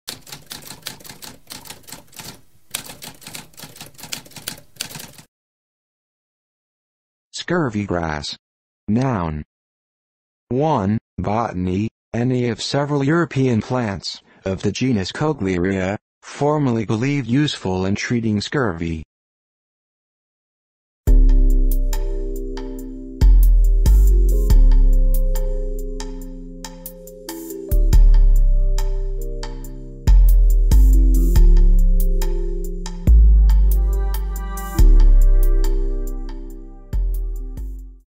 What does scurvy-grass mean?
A spoken definition of scurvy-grass.

Intro Sound:
Typewriter - Tamskp